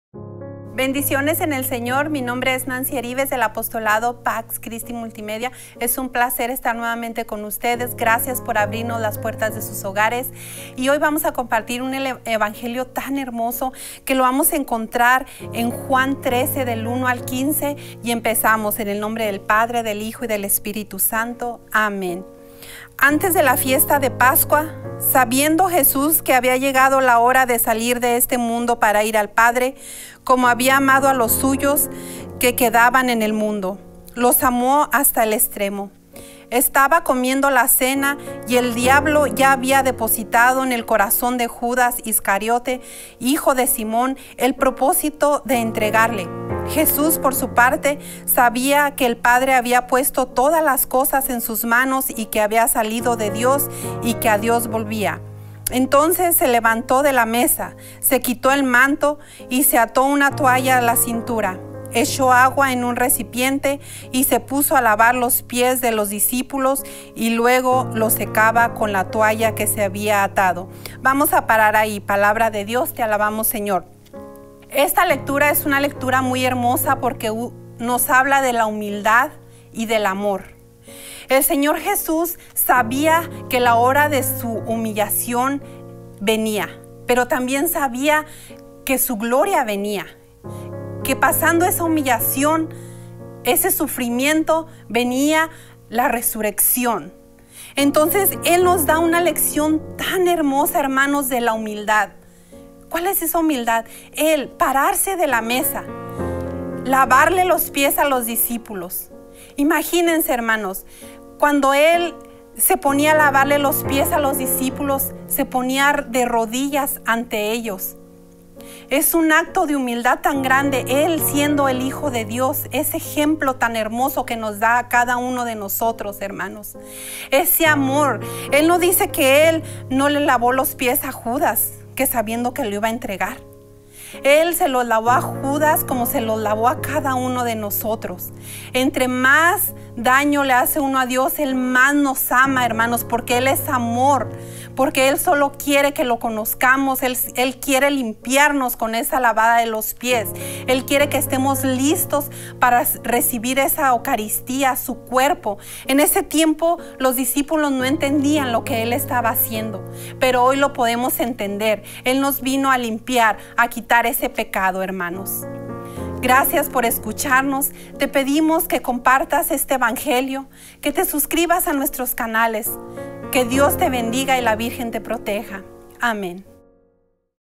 ⛪️ Evangelio del Día - 29 de Marzo de 2018 - Juan 13, 1-15 - Evangelio Explicado
Dinos por quién quieres que oremos, ofreceremos la Misa diaria y nuestra visitas al Santísimo Sacramento.